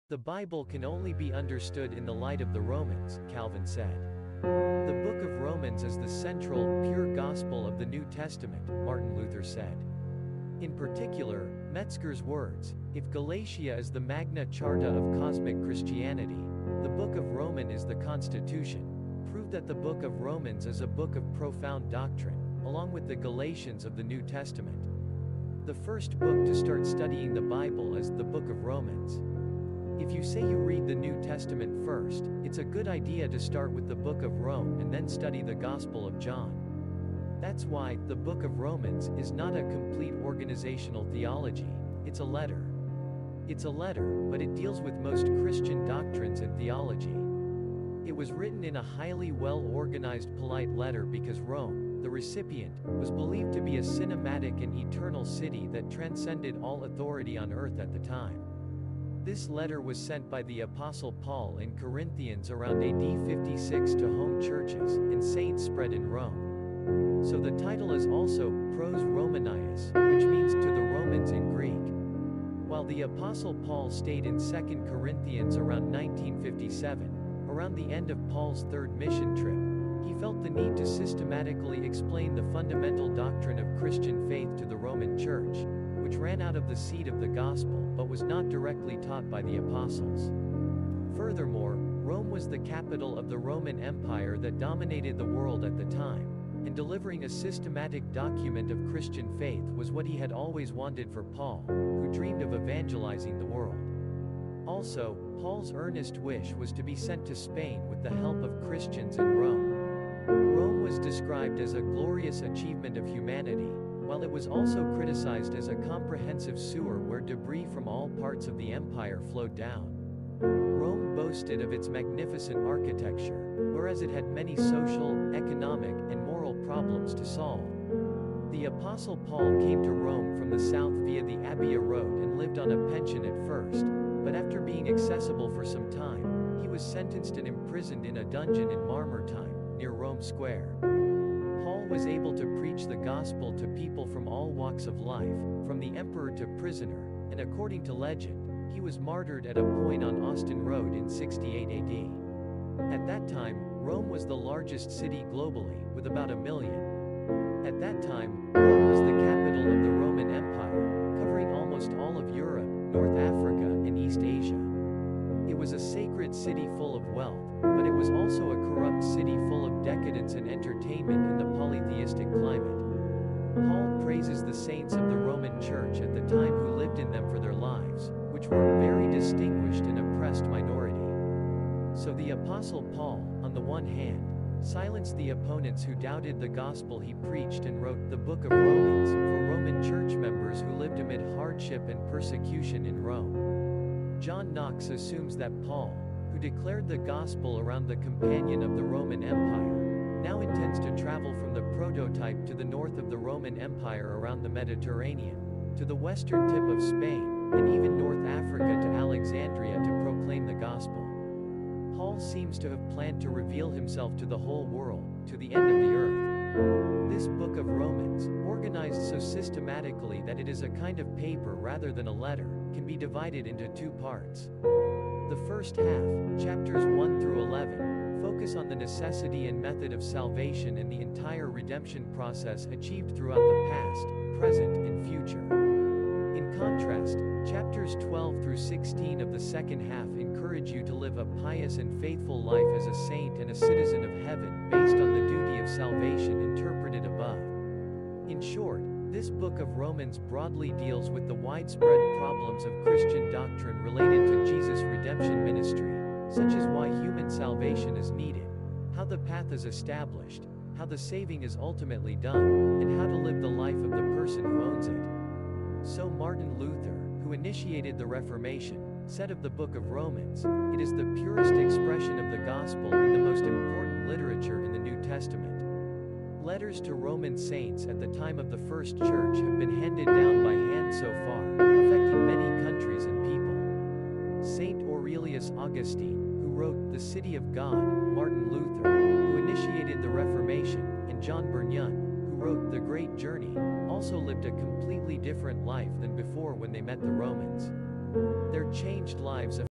Why the Book of Romans is Essential for Every Christian | Understanding Its Powerful Message
The Book of Romans stands as one of the most profound and influential texts in the New Testament.

In this video, we explore the significance of Romans, its key teachings on salvation, grace, and righteousness, and why it's a must-read for every believer.

Discover how this powerful letter from the Apostle Paul shapes our understanding of the gospel and lays the foundation for Christian faith.

Whether you're new to the Bible or a seasoned believer, Romans offers timeless wisdom that deepens your relationship with God. 📖

👉 Looking to grow in your faith?